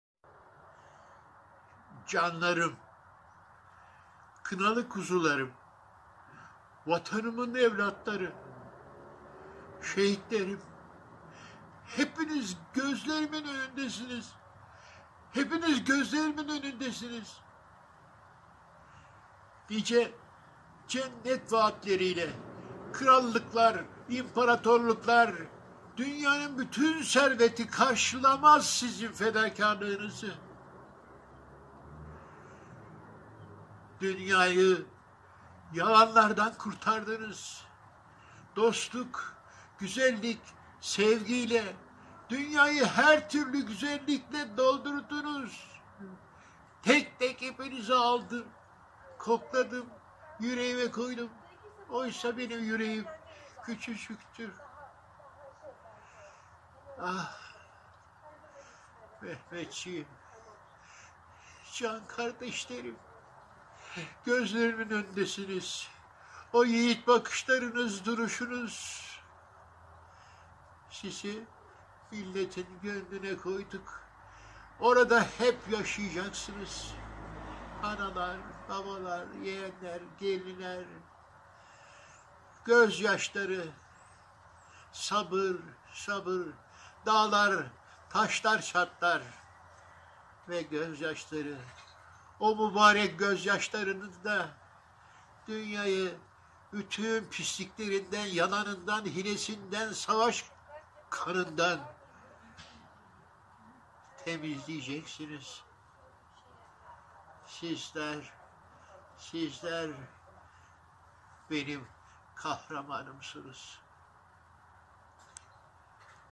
Cüneyt Arkın Şehitlerimiz İçin Ağladı
Ünlü oyuncu Cüneyt Arkın, İdlib'de şehit olan Türk askerleri için bir mesaj yayınladı. Arkın acısını dile getirirken gözyaşları içinde kaldı. İşte Cüneyt Arkın'ın o görüntüleri...